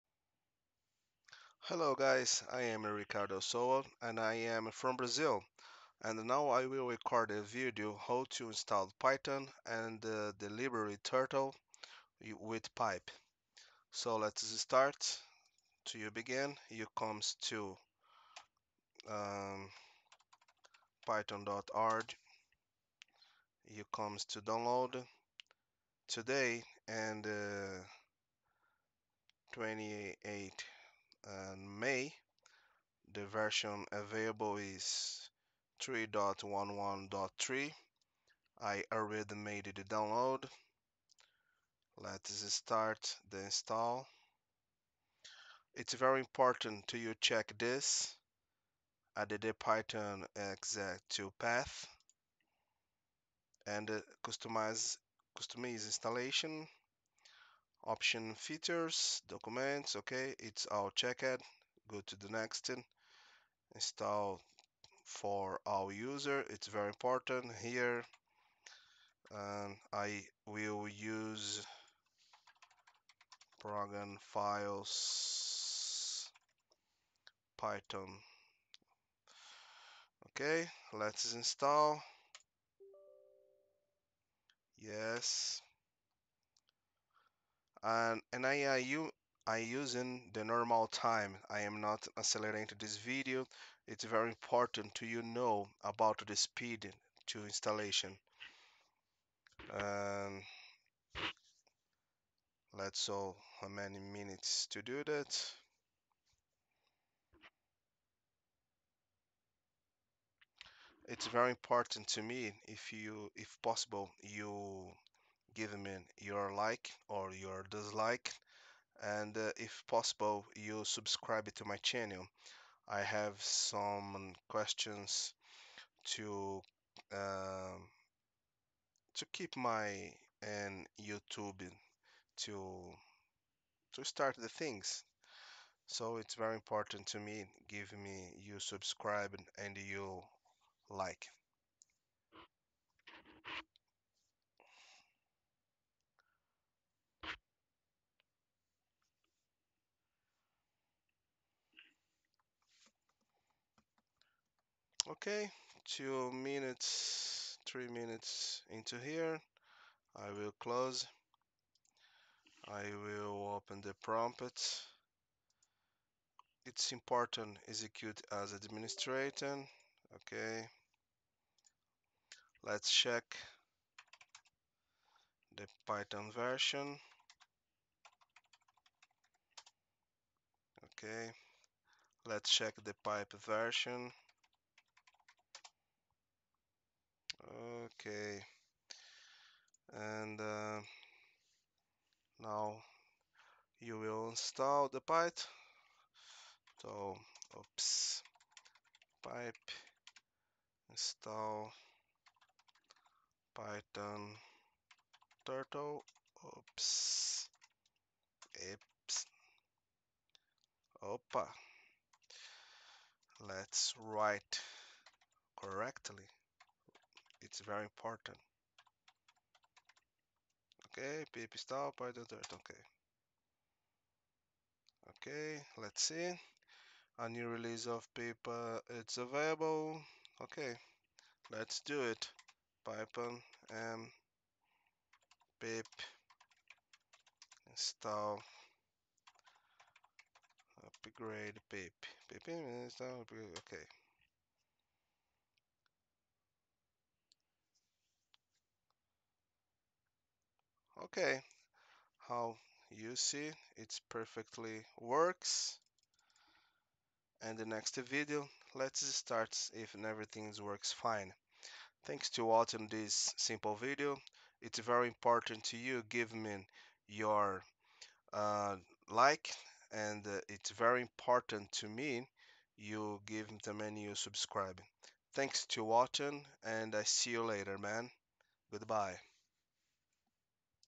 How to Install Python, PIP and Turtle Graphics in Windows 10/11 - English Version
In This video I will show you how to install Python, PIP and Turtle Graphics

Gratitude for your audience.